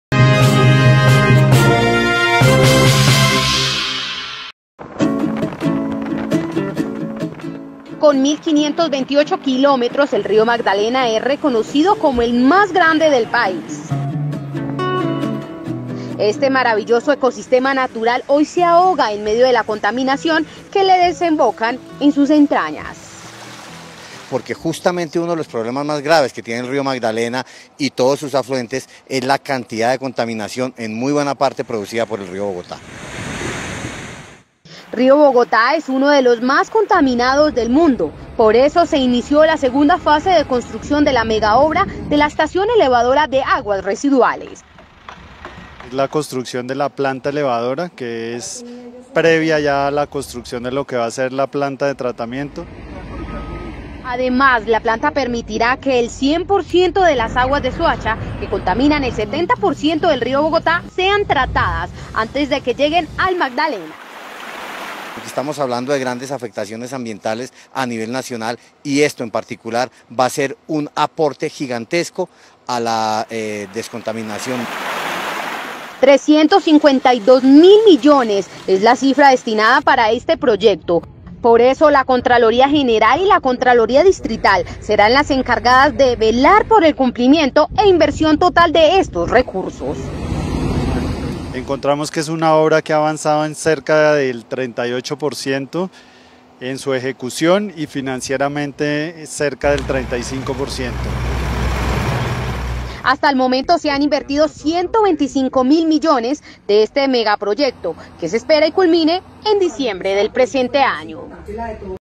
Río Magdalena dejará de recibir contaminación de Soacha
VÍA: DENUNCIAS MAS NOTICIAS / 12:36 PM